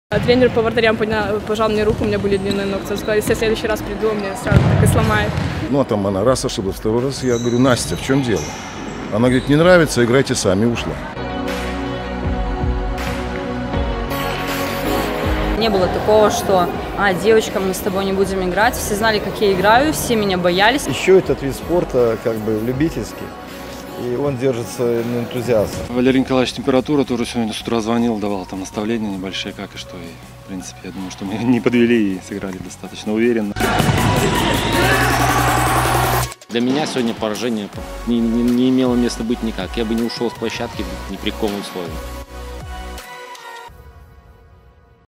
Ясно о мини-футболе #11 | "Дорожник" легко обыгрывает "УВД-Динамо" | Женский мини-футбол: как это?
- "Дорожник" без Досько не оставляет шансов "УВД-Динамо" при пустых трибунах
- Подтянули игроков минского Динамо и выиграли спартакиаду. БГУФК - лучшая женская  студенческая команда страны
- Маникюр у вратаря и особый подход тренеров: как стартовал женский чемпионат Беларуси (в трех матчах забили 35 (!!!) мячей)
- Школьный футбол. Когда на улице становится холоднее все перемещаются в залы.